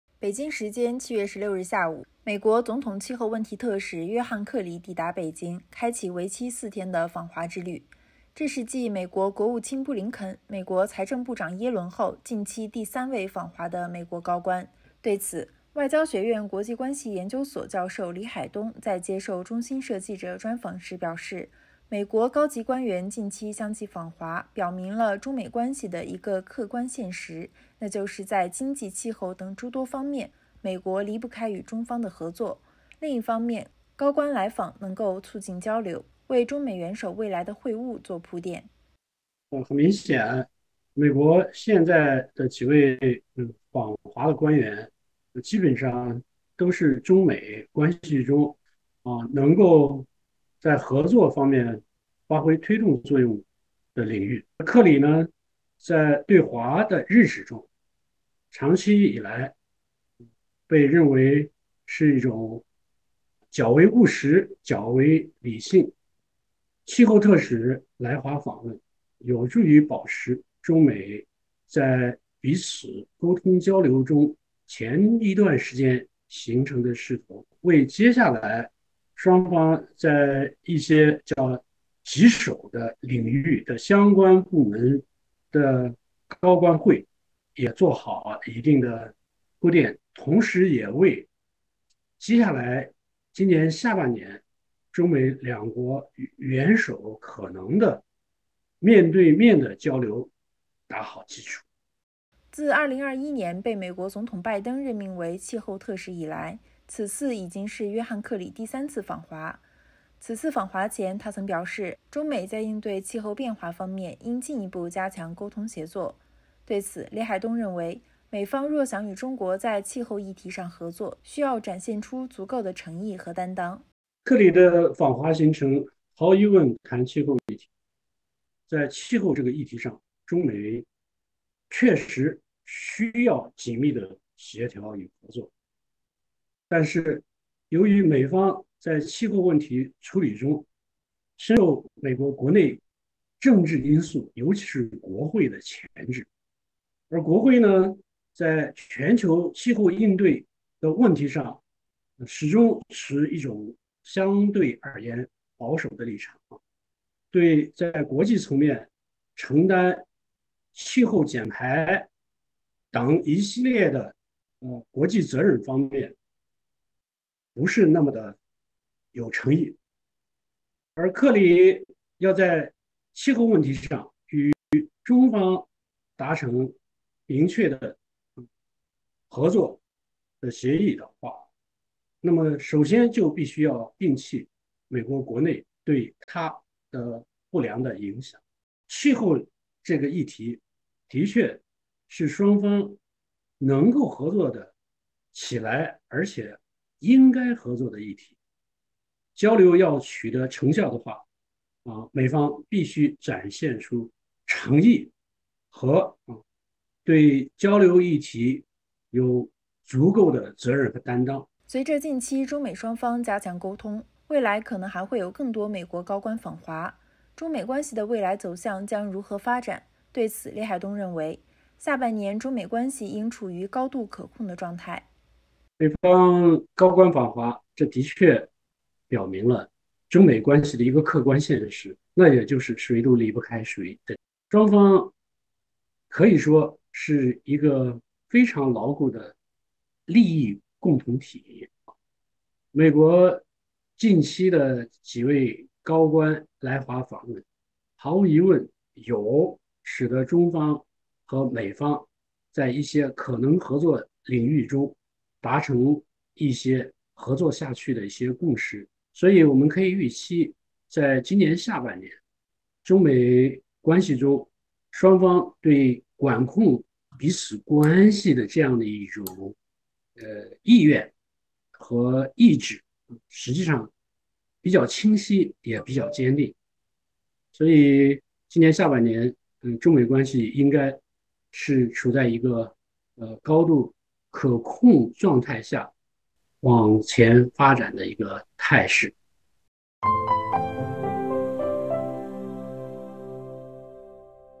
美国总统气候问题特使访华 专家：美方在气候议题合作方面需展现更多诚意
北京时间7月16日下午，美国总统气候问题特使约翰·克里抵达北京，开启为期4天的访华之旅。这是继美国国务卿布林肯、美国财政部长耶伦后近期第三位访华的美国高官。对此，外交学院国际关系研究所教授李海东在接受中新社记者专访时表示，美国高级官员近期相继访华，表明了中美关系的一个客观现实，那就是在经济、气候等诸多方面，美国离不开与中方的合作；另一方面，高官来访能够促进交流，为中美元首未来的会晤作铺垫。